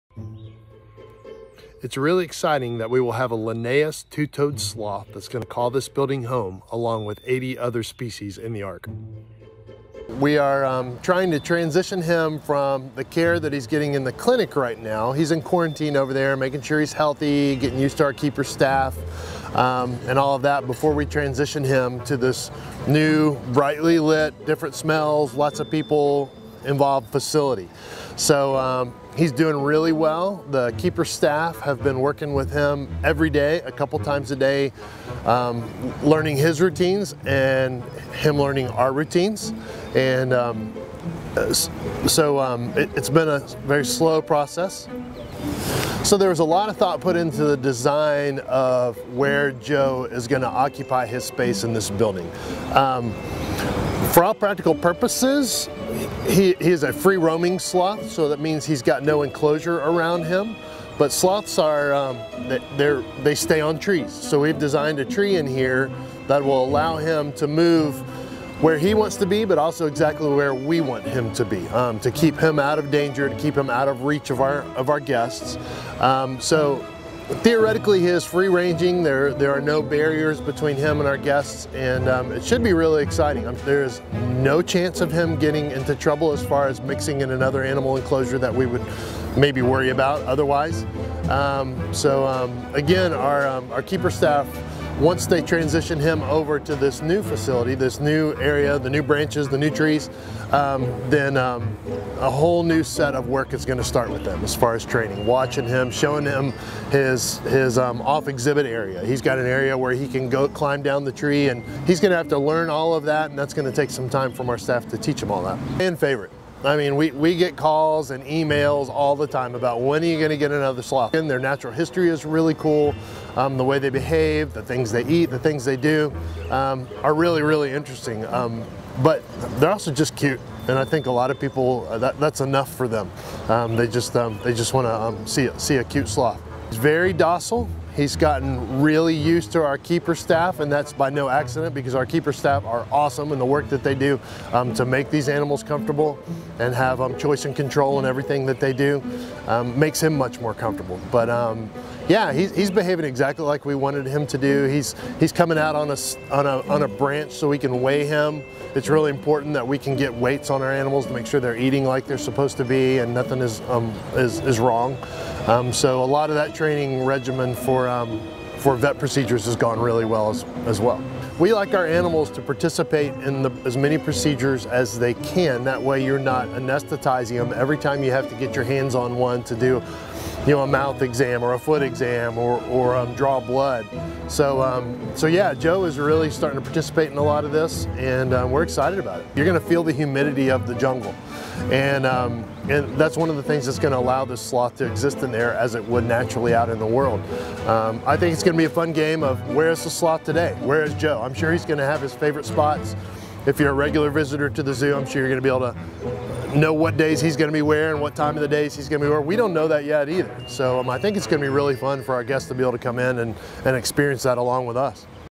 Meet Joe sloth, coming soon to Zoo Knoxville
Introducing the newest resident of the Clayton Family Amphibian and Reptile Conservation Campus, "Joe" the Linnaeus's two-toed sloth.